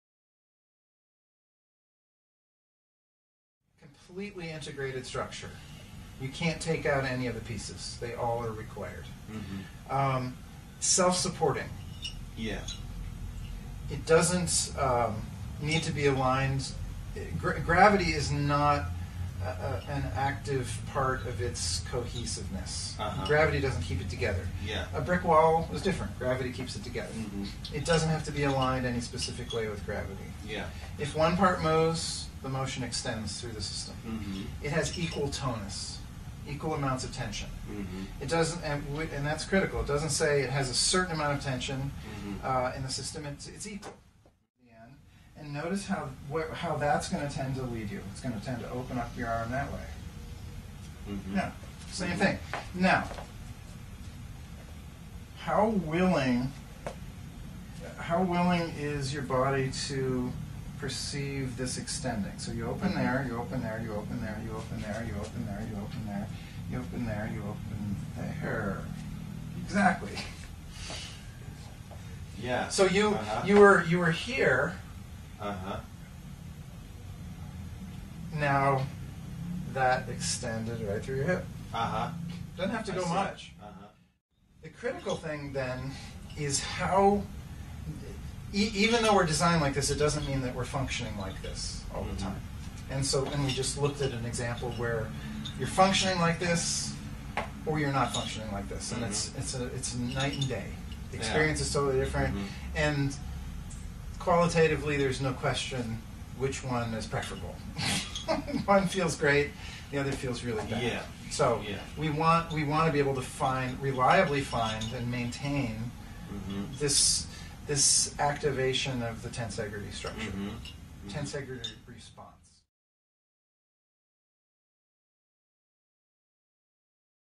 Biotensegrity with Doug Johnson (legendado)
Doug Johnson, professor da Berklee College, criador da *Kinematic Integration*,  explica noções básicas de biotensegridade.